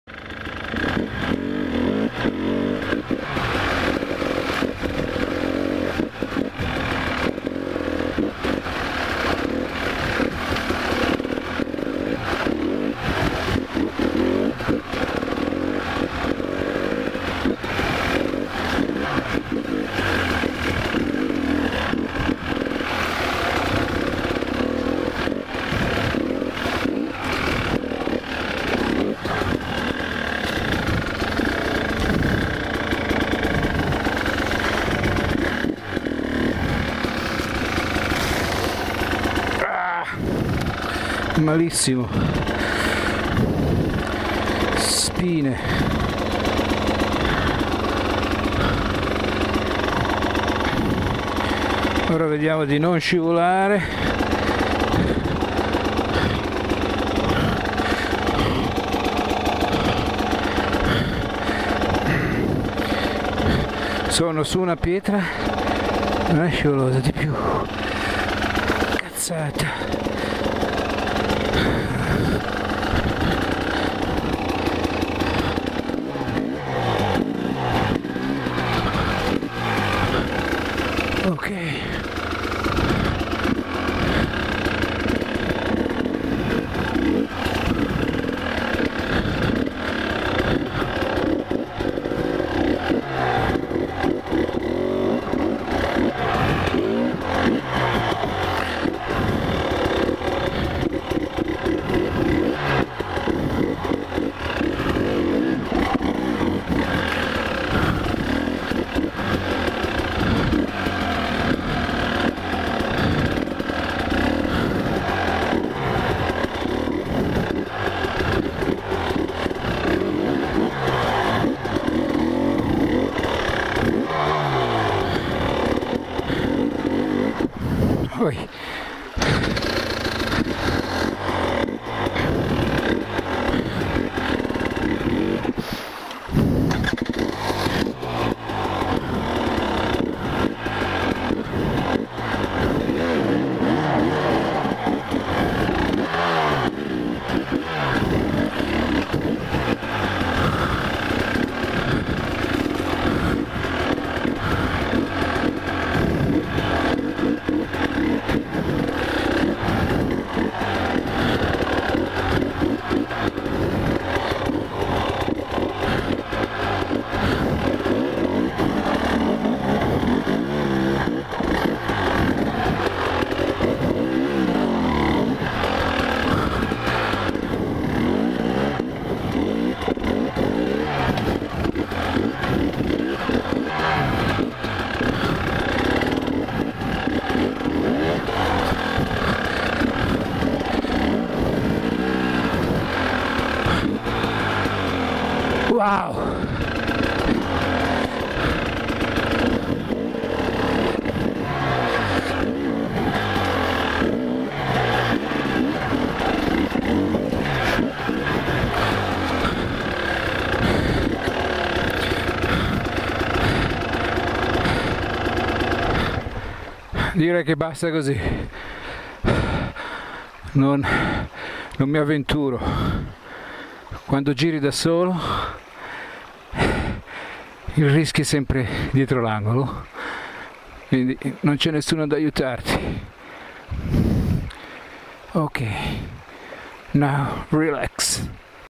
ENDURO - Krabi enduro ride. December 6th 2020 Figthing with the river.
Video from Giancarlo